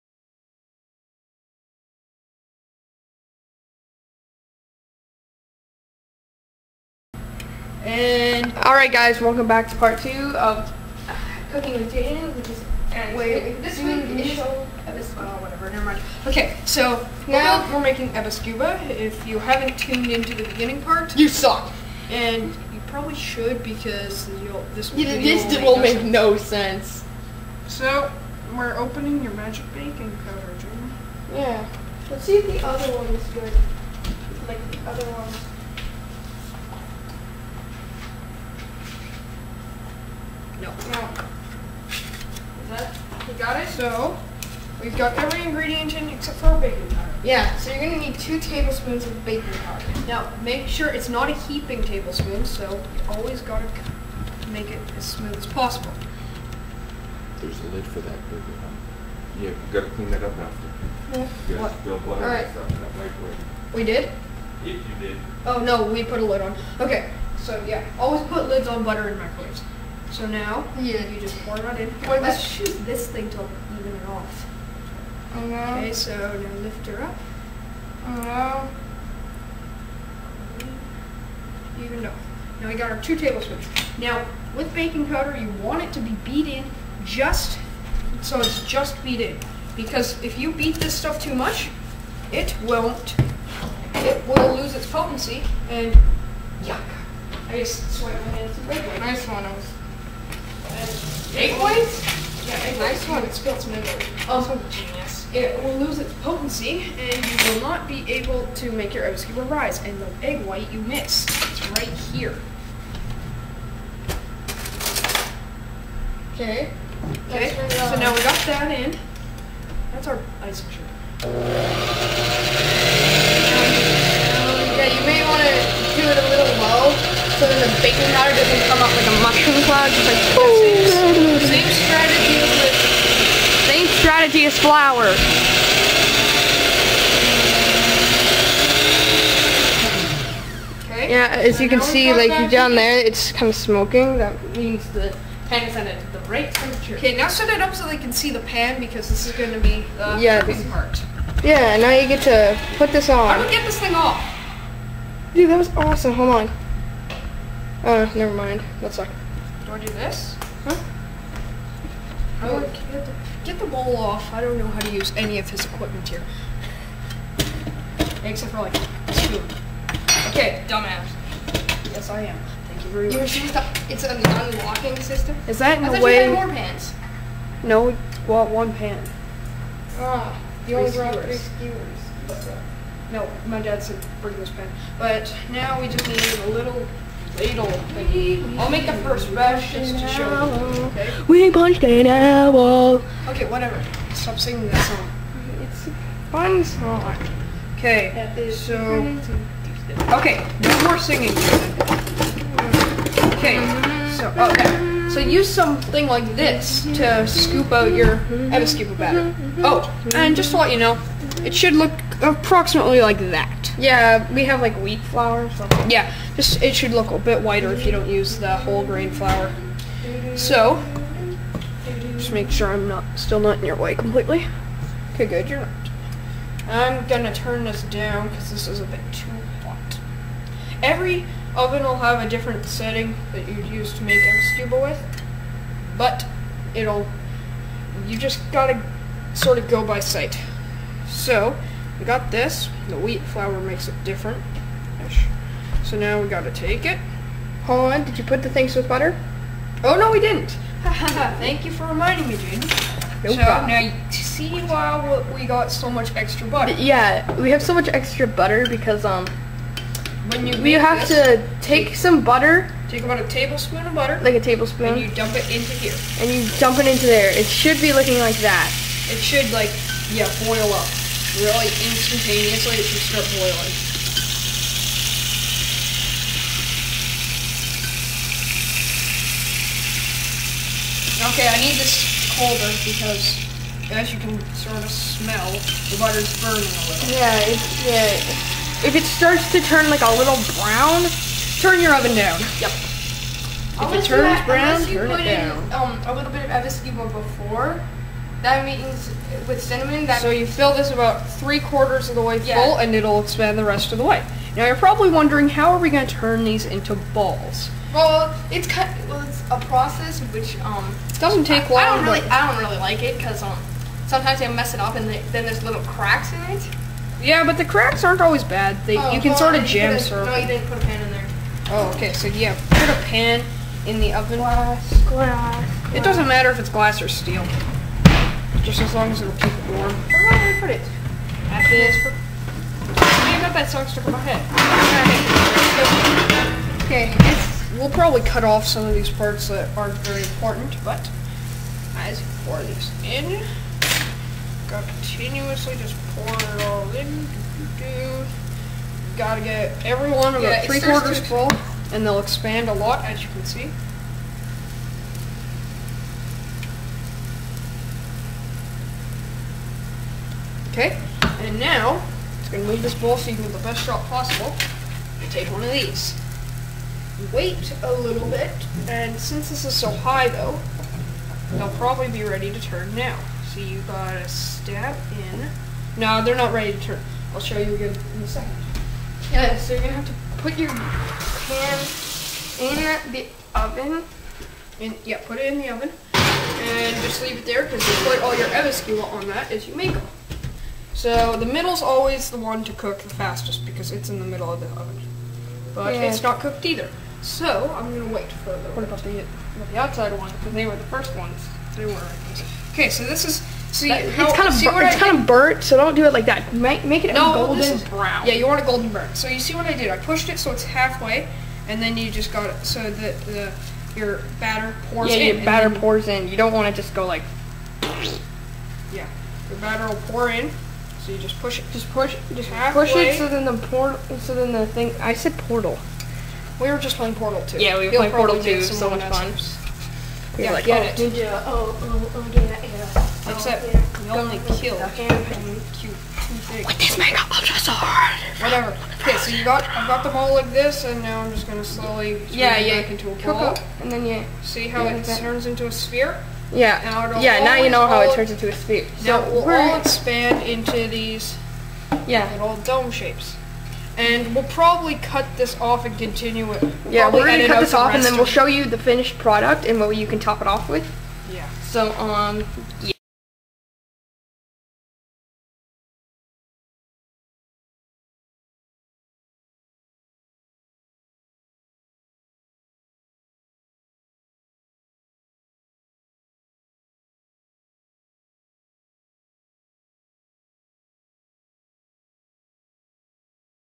Cooking with Jayden episode 1 part 2 of 3 aebleskiver (pancake)
This is the second part of my first episode. Keep checking for the video about the videos coming soon. The next cooking with Jayden episode will be orange smoothies which will be in 1 video.